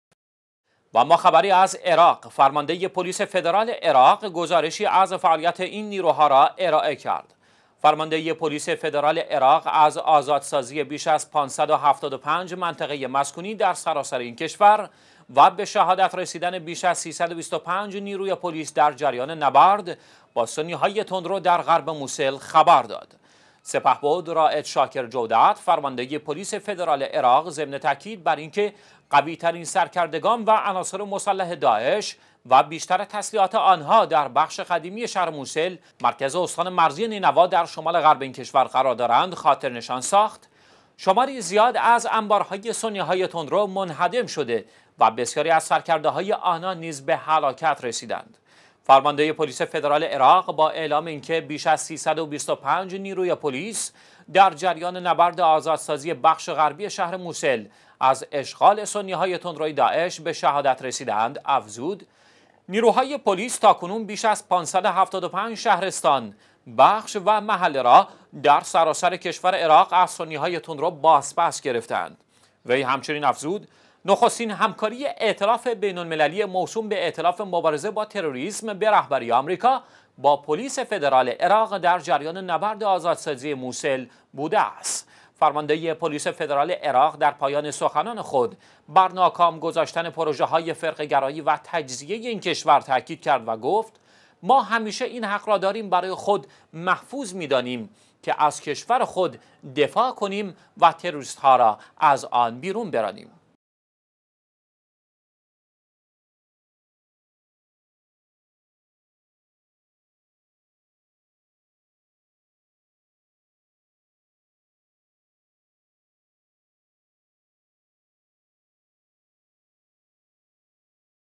گزارش فرمانده پلیس فدرال عراق درباره فعالیت این نیروها / ۱۳ فروردین ۱۳۹۶
فرمانده پلیس فدرال عراق گزارشی از فعالیت این نیروها ارائه کرد.

فرمانده پلیس فدرال عراق از آزادسازی بیش از ۵۷۵ منطقه مسکونی در سراسر این کشور و به شهادت رسیدن بیش از ۳۲۵ نیروی پلیس در نبرد غرب «موصل» توسط سنی های تندرو، خبر داد.

سپهبد «رائد شاکر جودت» فرمانده پلیس فدرال عراق، ضمن تاکید بر این که قوی ترین سرکردگان و عناصر مسلح داعش و بیشتر تسلیحات آن در بخش قدیمی شهر «موصل» ـ مرکز استان مرزی نینوا در شمال غرب این کشور ـ قرار دارند، خاطر نشان ساخت که شمار زیادی از انبارهای سنی های تندرو منهدم شده و بسیاری از سرکرده های آنان نیز به هلاکت رسیدند.

فرمانده پلیس فدرال عراق با اعلام این که بیش از ۳۲۵ نیروی پلیس در جریان نبرد آزادسازی بخش غربی شهر موصل از اشغال سنی های تندروی داعش به شهادت رسیده اند، افزود: نیروهای پلیس تاکنون بیش از ۵۷۵ شهرستان، بخش و محله را در سراسر کشور عراق از سنی های تندرو، بازپس گرفته اند.

وی همچنین افزود: نخستین همکاری ائتلاف بین المللی، موسوم به ائتلاف مبارزه با تروریسم به رهبری آمریکا، با پلیس فدرال عراق، در جریان نبرد آزادسازی «موصل» بوده است.

فرمانده پلیس فدرال عراق در پایان سخنان خود، ضمن تاکید بر این که پیش از این پروژه های فرقه گرایی و تجزیه این کشور را ناکام گذاشته اند، خاطر نشان کرد: ما همیشه این حق را برای خود محفوظ می دانیم که از کشور خود دفاع نموده و تروریست ها را از آن بیرون برانیم.

یکشنبه - ۱۳ فروردین ۱۳۹۶
الأحد - ٤ رجب ١٤٣٨